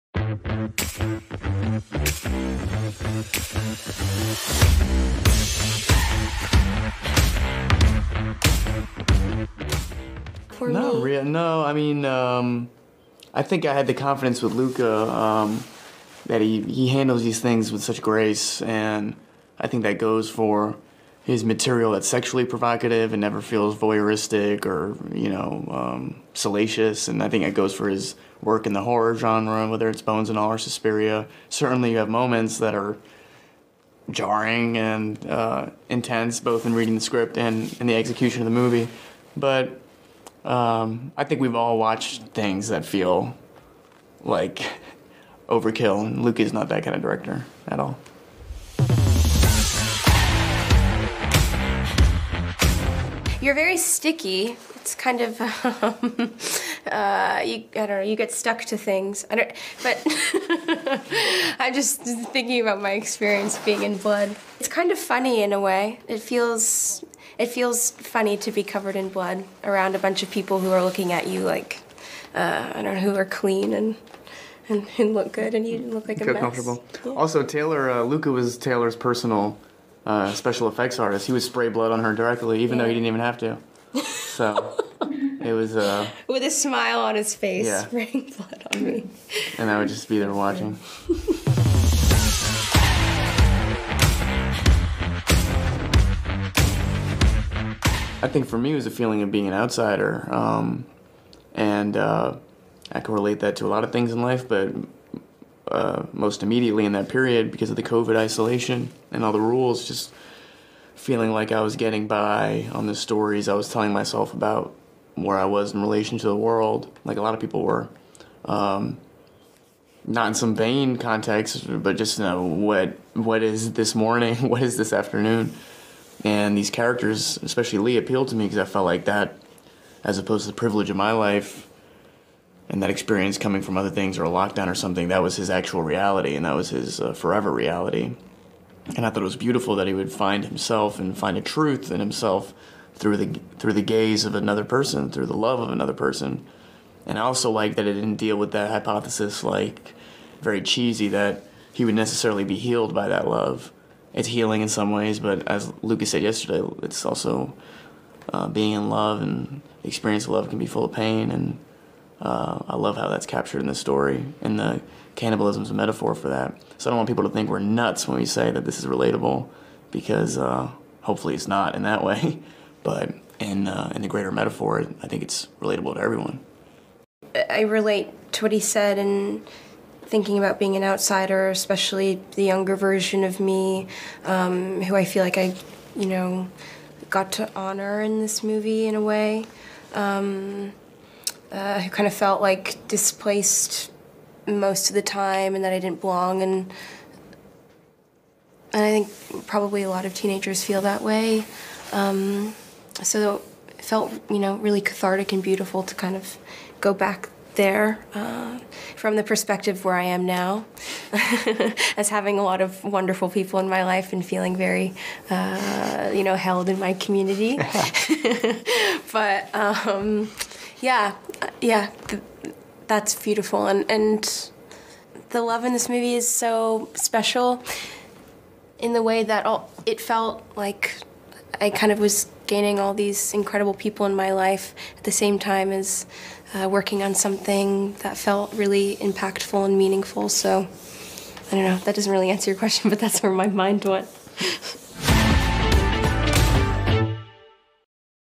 Тимоти Шаламе и Тейлор Расселл про любовь, метафоры и съемки фильма "Целиком и полностью" 2022
All about Timothée Chalamet (Тимоти Шаламе)
•Переводим интервью и статьи
•Освещаем церемонии, премии и выходы